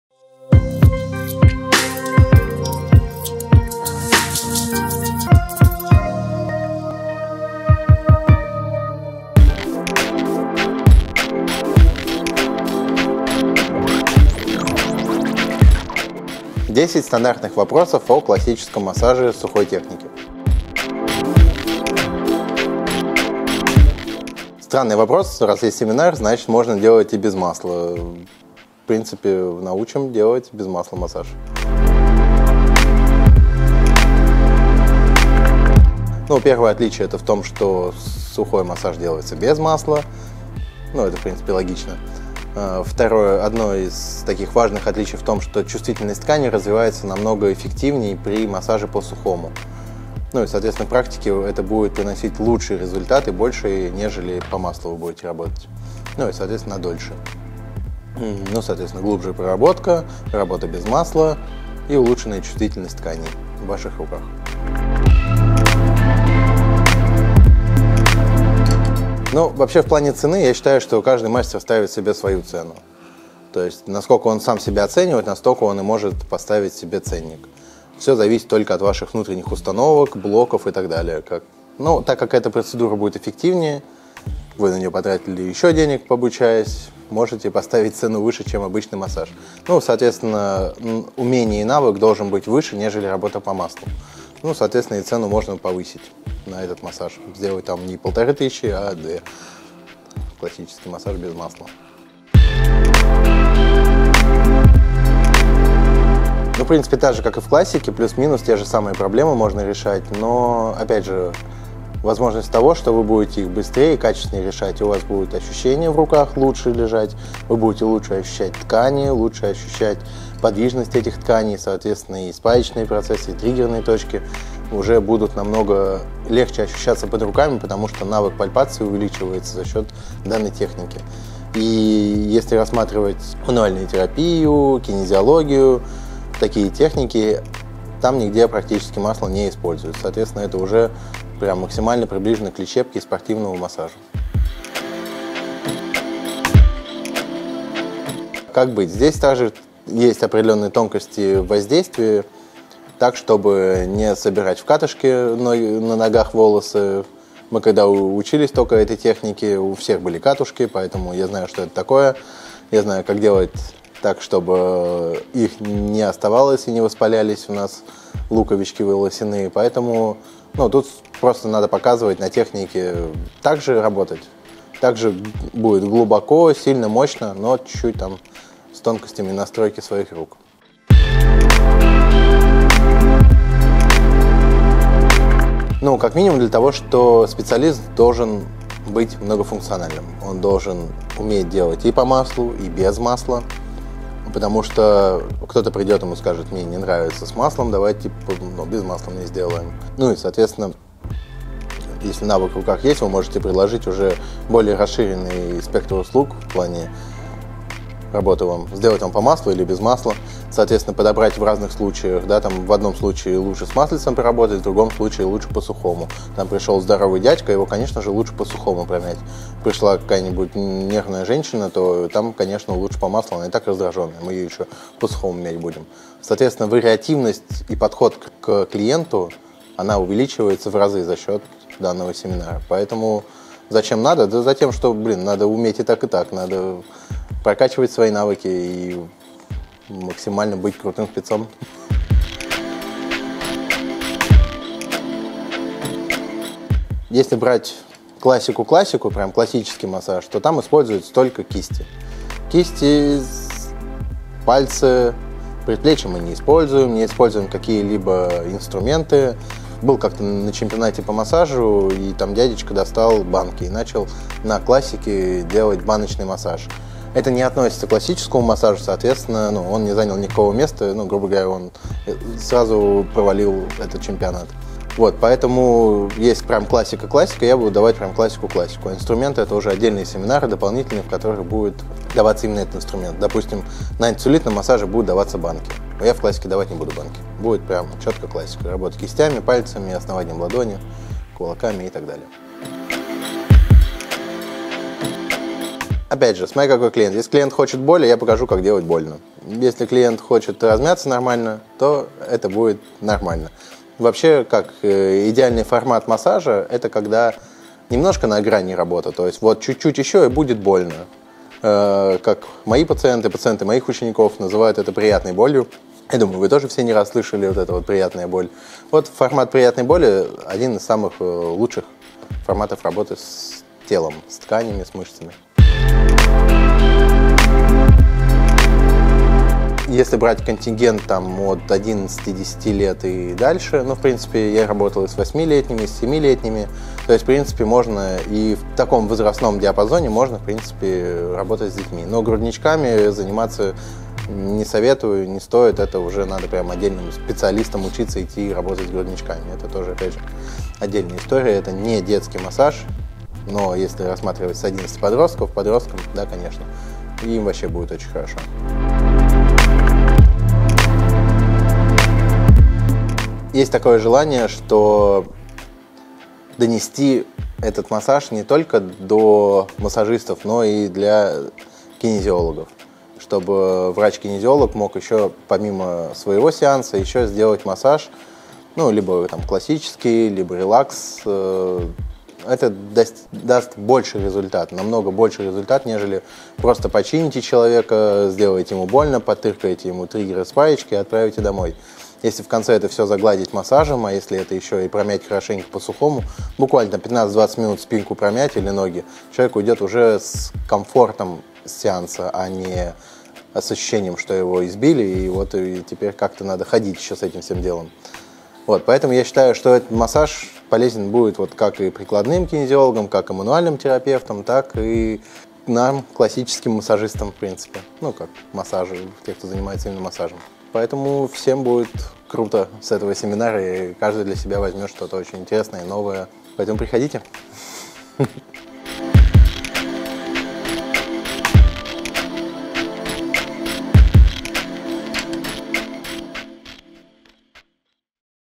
10 вопросов: Сухой классический массаж
0:22 Можно ли делать массаж без масла?
0:32 Чем отличается от обычной классики?
1:07 Сколько будет стоить сеанс?
1:42 Какие проблемы я смогу решать этим видом массажа?
2:27 Как быть с волосатыми мужчинами?
3:04 Зачем мне изучать сухую классику, если я знаю обычный масляный массаж?
4:04 Этот массаж делают только руками или с использованием инструментов?
4:54 Сухая классика - это больно?
5:29 Можно ли делать такой массаж детям?
5:59 Кому будет полезен этот семинар?